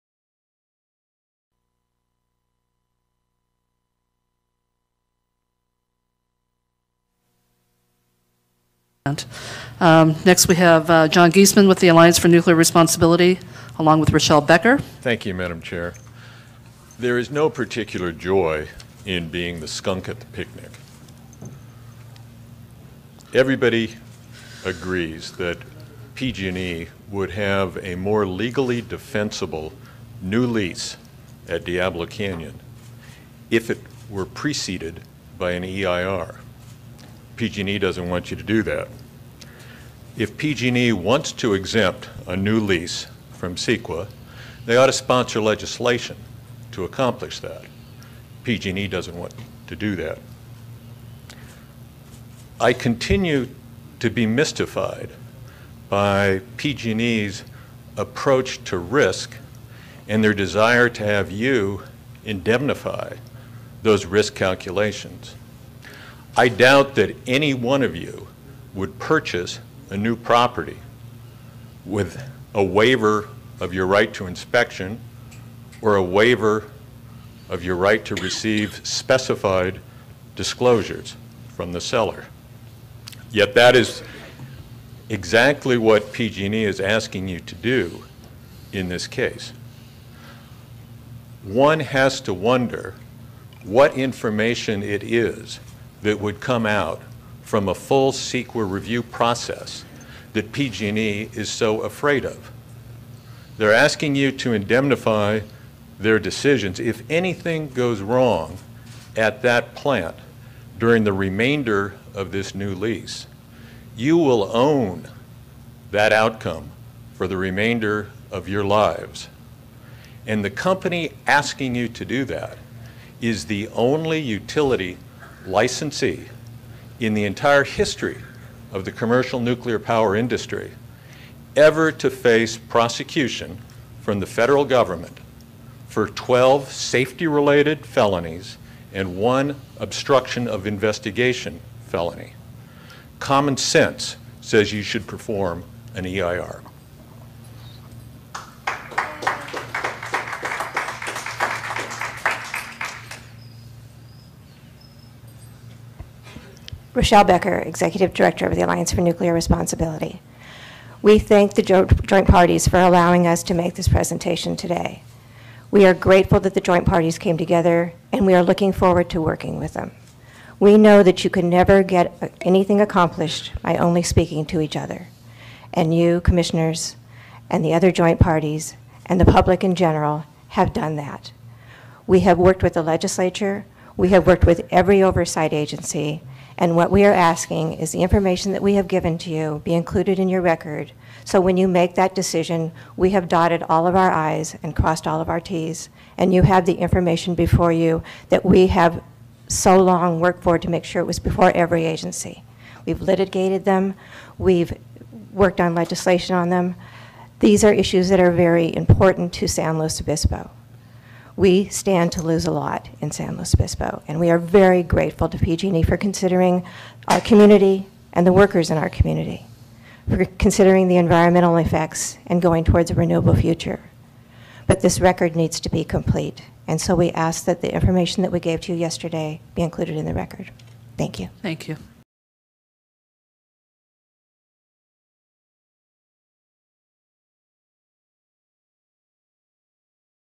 Geesman & Becker on Diablo Shutdown Proposal 6-28-2016
Attorney John Geesman and Executive Director Rochelle Becker of the Alliance for Nuclear Responsibility (A4NR.org) speak at the July 28, 2016 State Lands Commission meeting about the Joint Proposal to shut down Diablo Canyon at the end of nine years. The Alliance is one of the Parties in the Joint Proposal with PG&E.